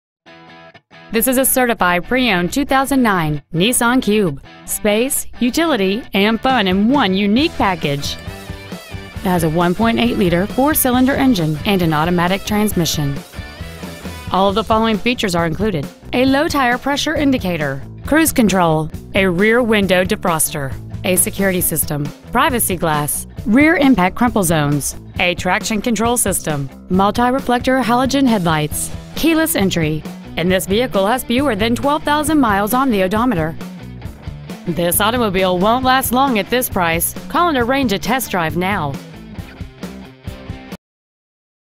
2009 Nissan cube Long Beach CA
We are proud to present this 2009 Nissan cube .

 Year : 2009

 Make : Nissan

 Model : cube

 Engine : 4-Cyl 1.8 Liter

 Trans . : CVT Automatic

 Exterior : Sky Blue

 Miles : 11,160

 Interior : Beige

 30 Auto Center Drive

 2009 Nissan cube Irvine CA